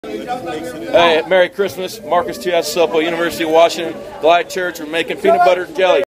Washington's Coach Tuiasosopo sends fans message from Glide Memorial Church
While preparing meals, Washington's Marques Tuiasosopo sends an update to followers, explains what they are doing at Glide Memorial Church. He and the team helped create Christmas miracles one meal at a time.